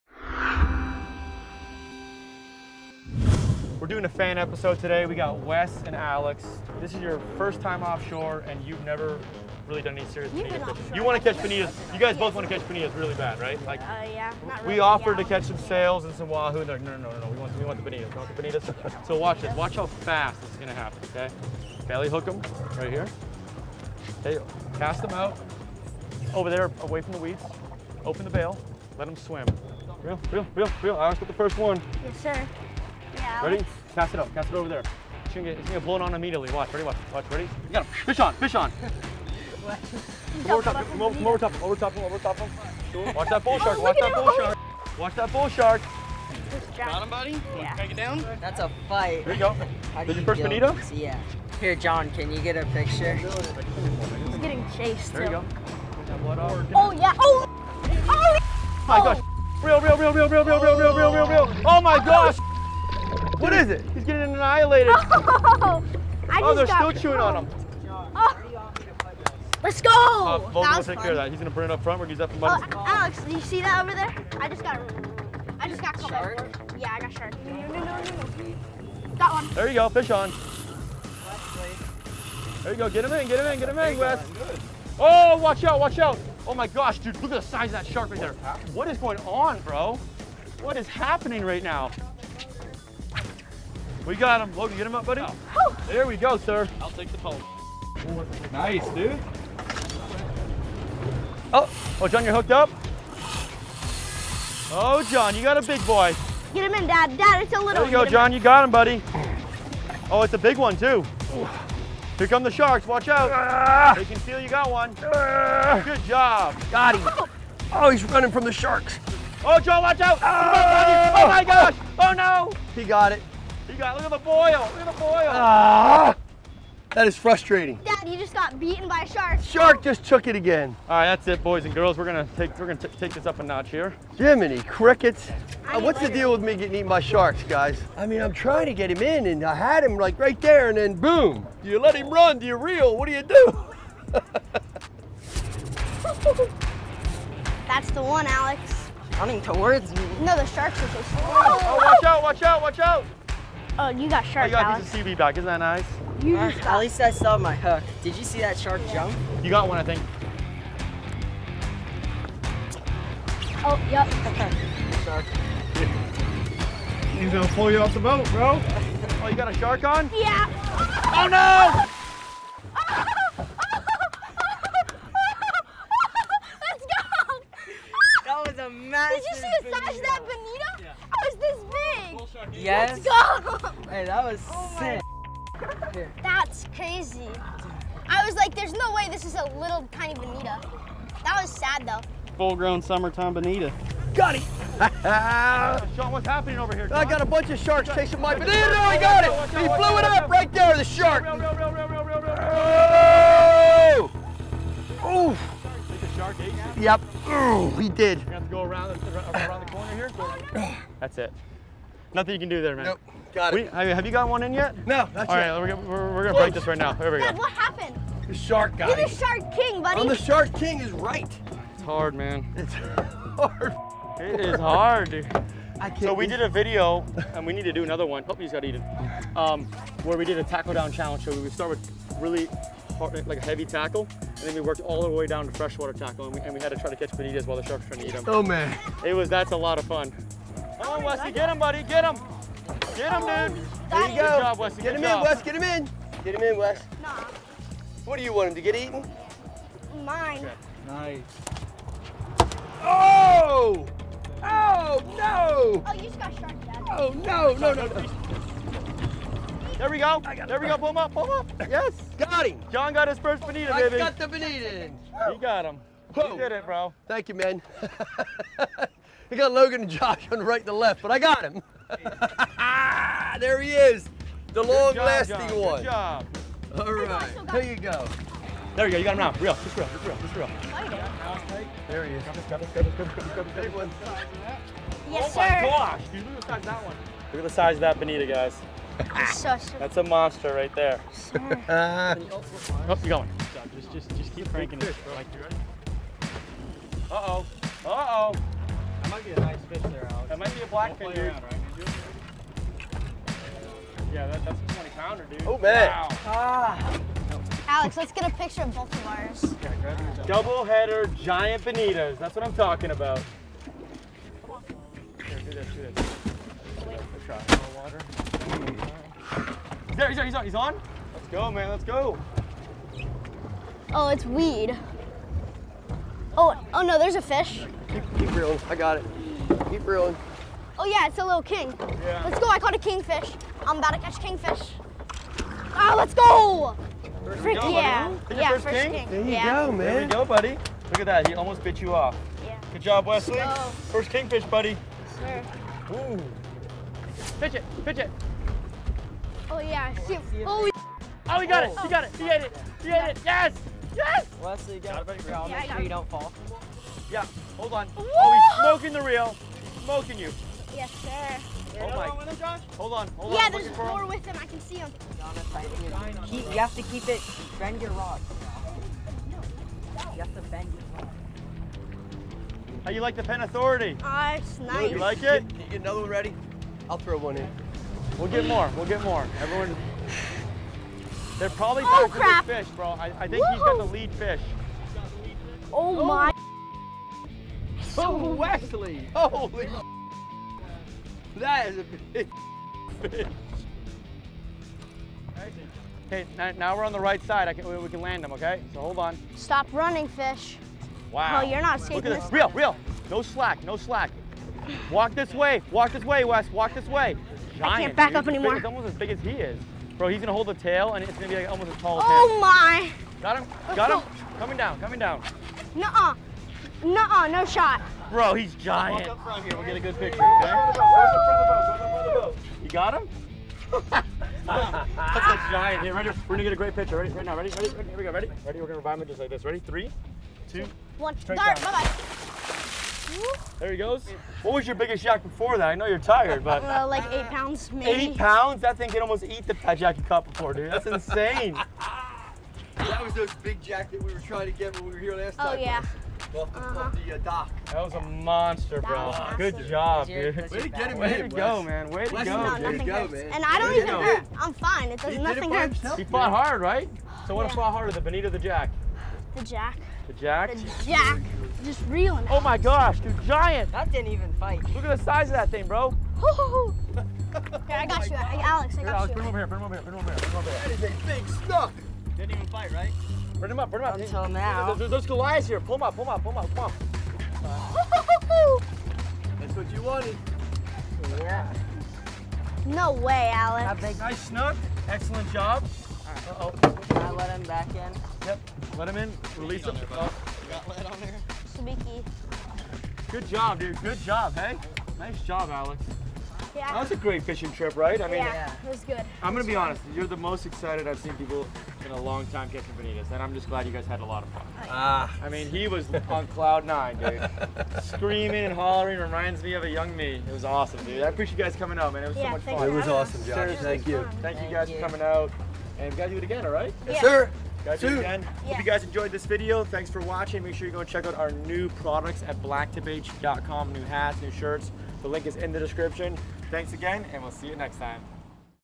These Fish were Almost Impossible to Catch
In this episode of Blacktiph, it was almost impossible to catch these fish! We began by hooking bonitos and fighting them past the shark and to the boat. John, Wes, and Alex lost their first fish to the sharks. After a while, they finally started getting them in the boat, for we kept a few and then headed to the next spot, where we caught a smaller kingfish. On our way back to the boat ramp, we found a school of jack crevalles swimming down the beach. Wes cast a bait to them and the biggest one at it- he was a giant! We then dropped a bait to the bottom and finished the day off with Alex catching a giant snook!

▬▬▬▬▬▬▬ ABOUT BLACKTIPH ▬▬▬▬▬▬▬

▬▬▬▬▬▬▬ BLACKTIPH PRODUCTS ▬▬▬▬▬▬▬

▬▬▬▬▬▬▬ MUSIC SOURCES ▬▬▬▬▬▬▬

• Pond5
• Audio Jungle
• PremiumBeat

▬▬▬▬▬▬▬ PREVIOUS EPISODE ▬▬▬▬▬▬▬

Fishing in Belize!!